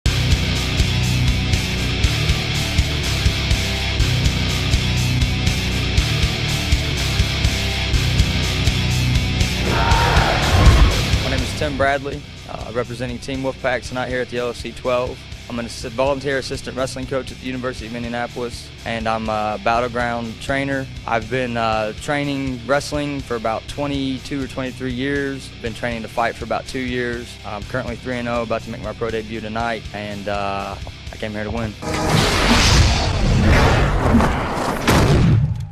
Tim Bradley
Tim is making his pro debut New Years Eve at LFC 12